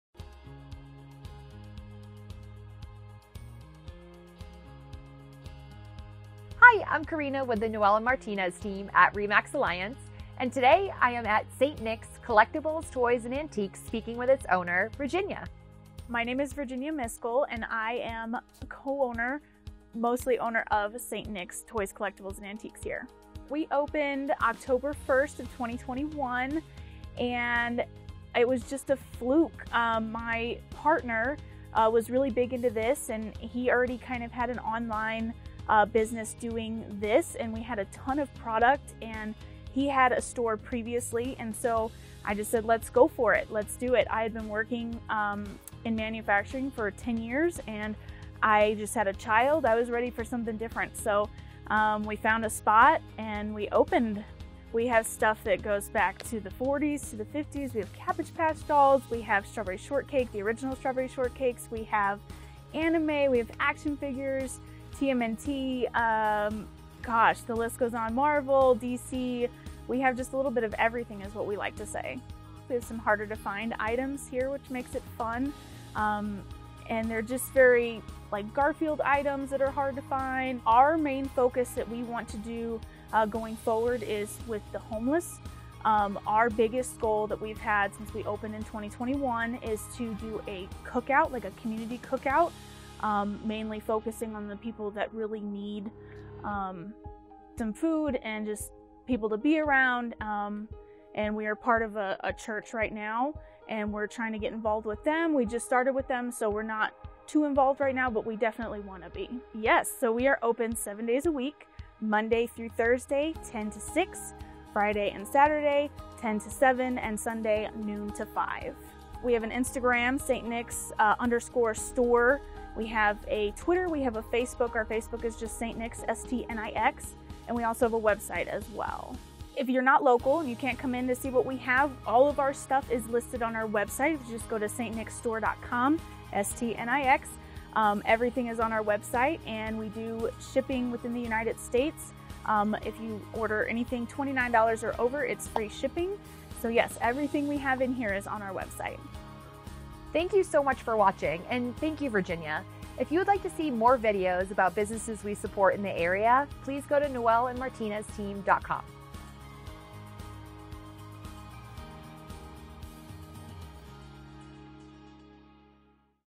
St Nix Collectibles Toys and Antiques
St Nix Collectibles Toys and Antiques is located in Downtown Longmont.  Opened in Oct of 2021, as a family owned business. They have a little bit of everything and hard to find items. Their main focus is with the homeless and to support them with their Church.  Open 7 Days a week.